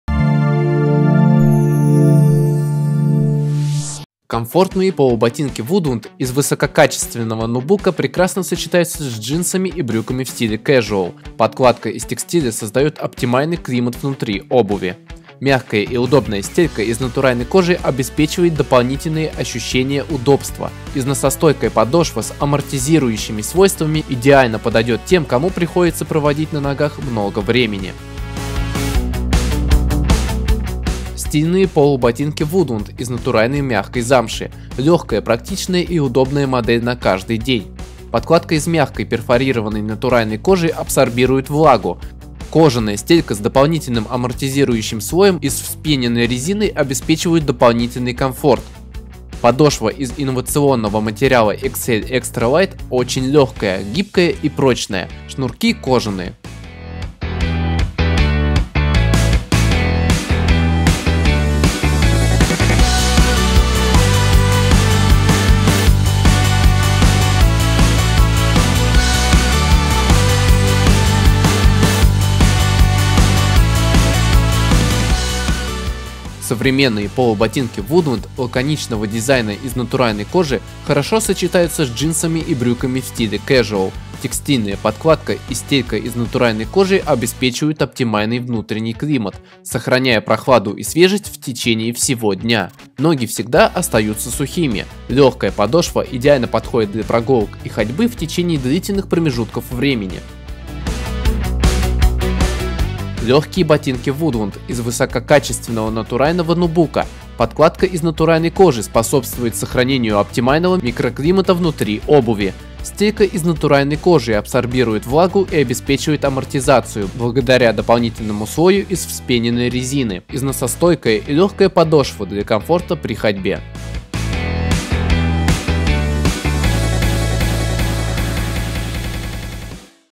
Мужские ботинки и полуботики Woodland
Презентация достоинств обуви бренда Woodland (ботинки, полуботинки).

Видеопродакшн. Цикл работ по продвиджению и развитию брендов